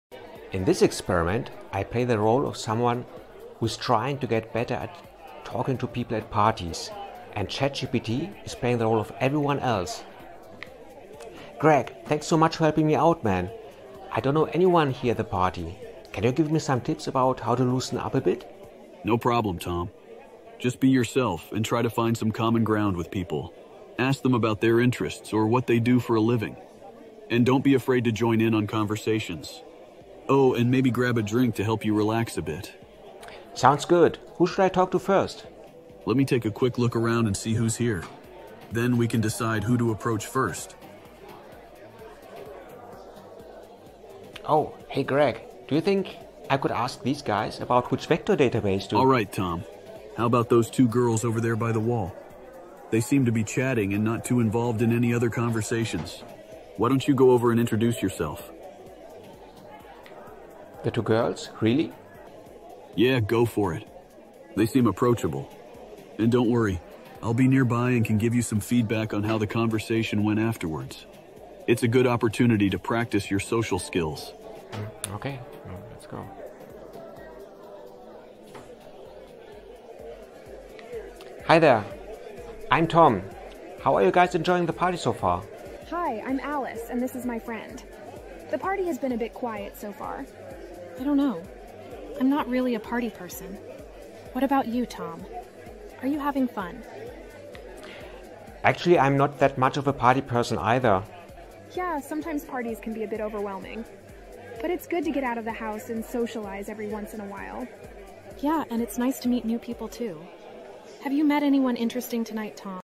Chatting with NPCs at a Virtual Party!
Youtuber ''Tamulur'' did this experiment using OpenAI's Whisper for speech-to-text, GPT3.5-Turbo for the NPC's "brains", and ElvenLabs for text-to-speech.

The NPC was giving him advices on how to talk to other people in the party!